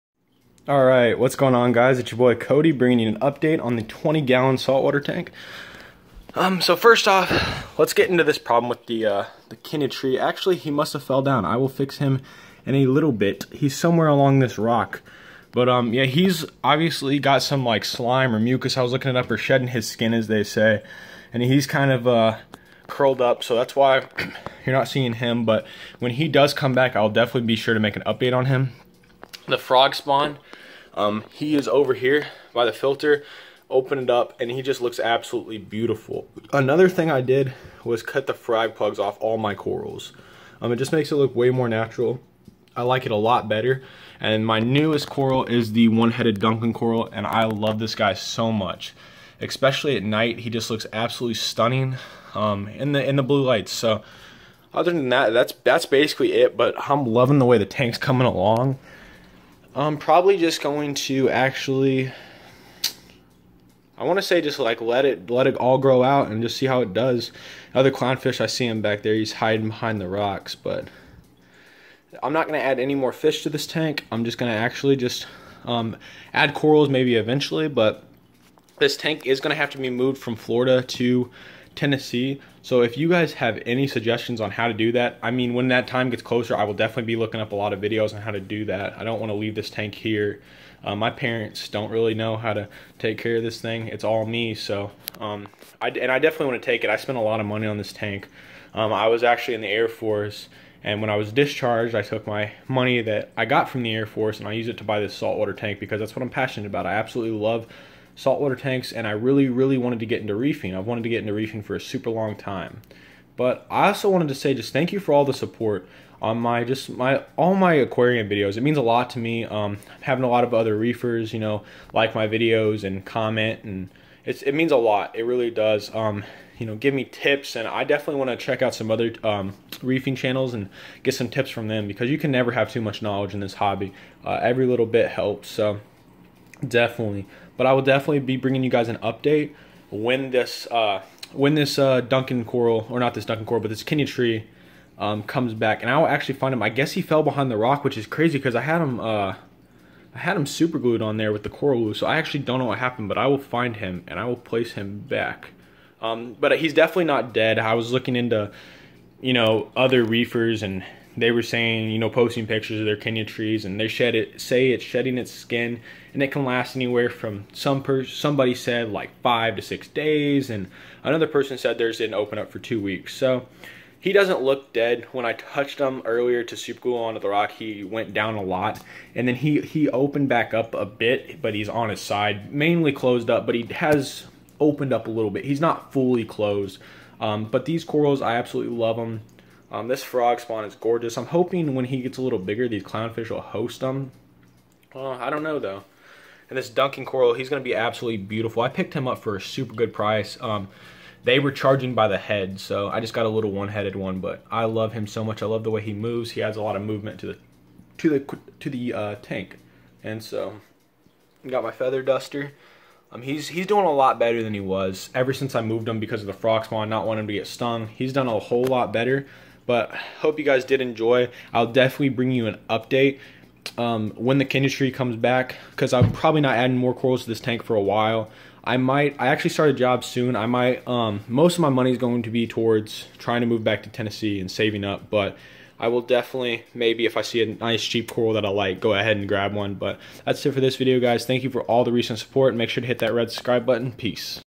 20 gallon reef update/ New Duncan Coral
Found the Kenya tree the rock had rotated and he was in the back so moved the rock back around